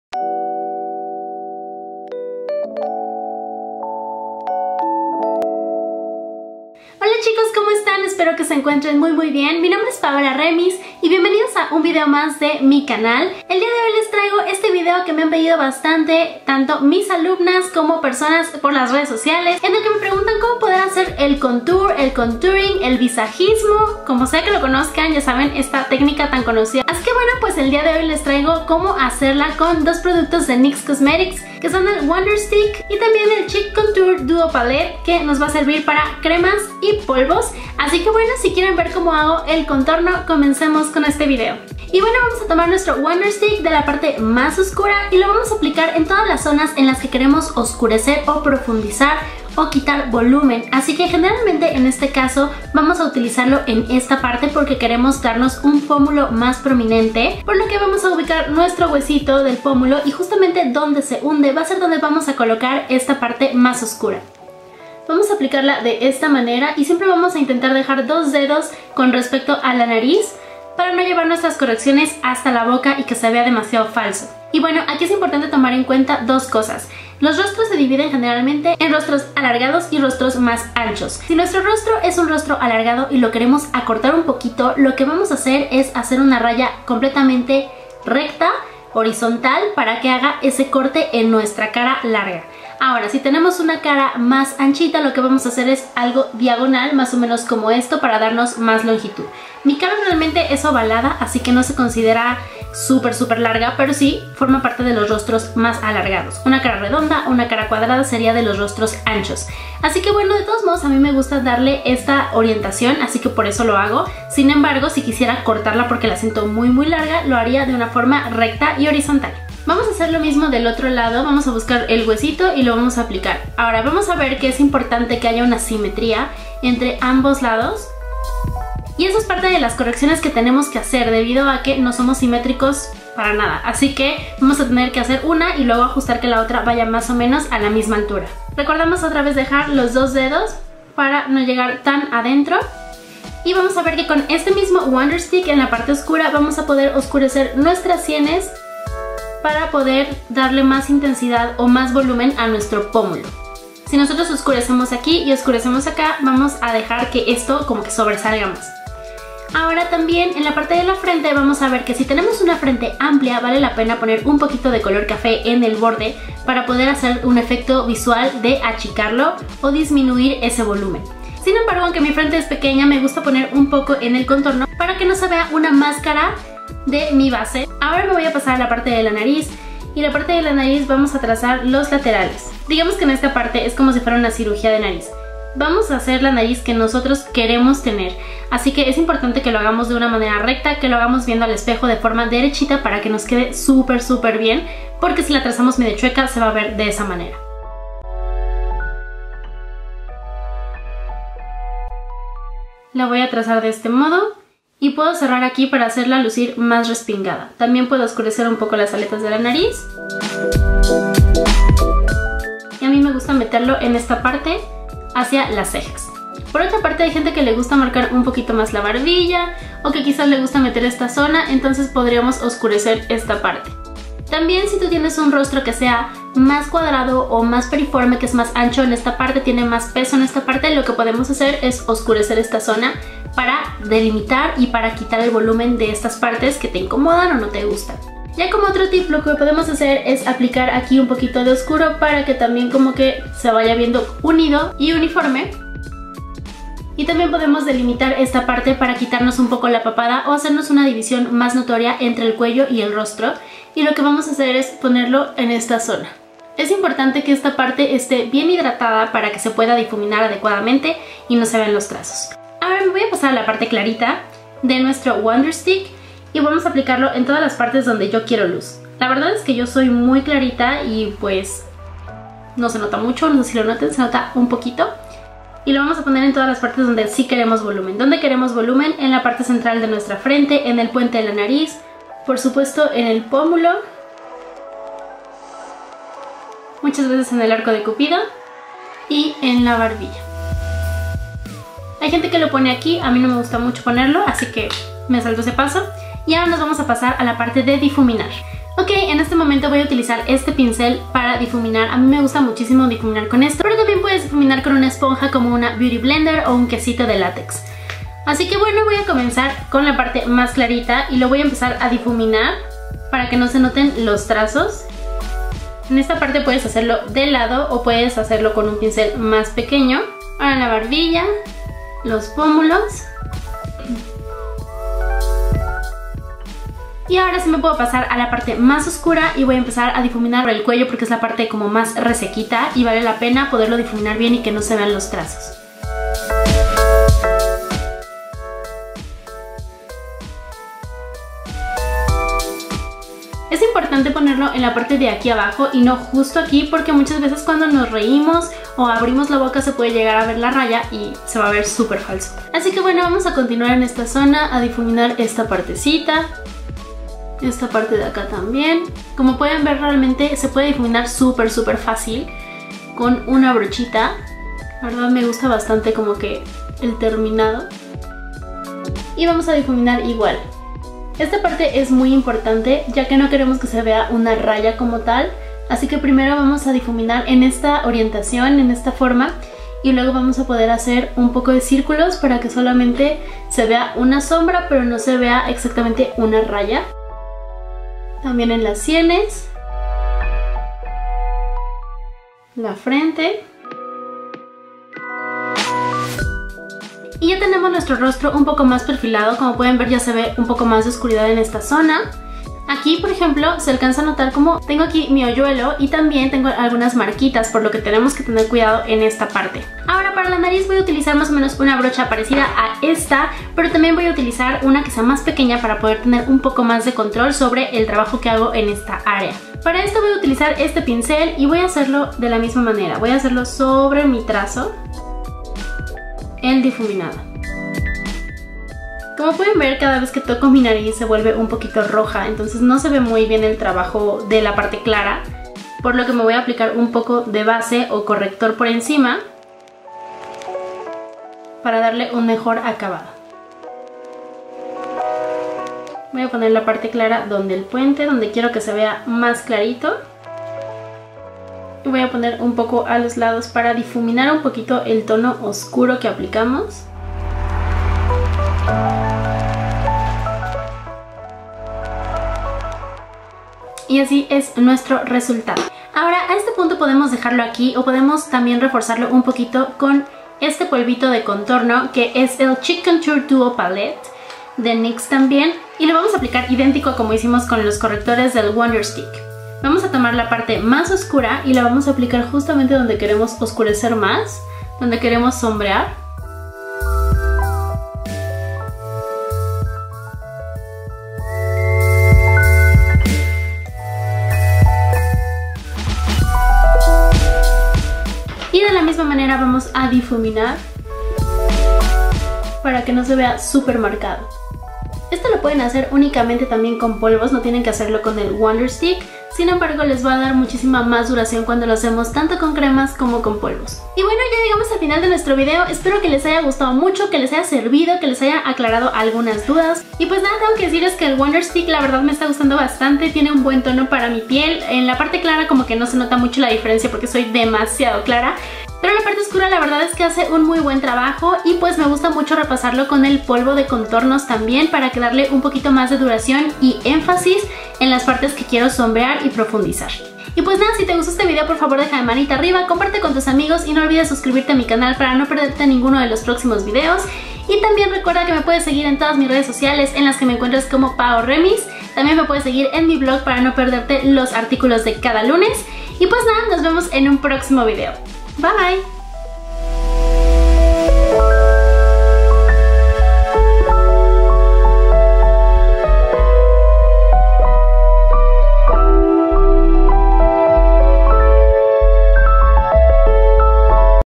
CONTOURING (PASO A PASO) - PAO REMIS
Hola hola!! El día de hoy les dejo este videito en el que les enseño paso a paso como hacer visagismo o contour con 2 productos de NYX Cosmetics. Espero les guste mucho!!

♡ Cambia la visualización del video a Full HD.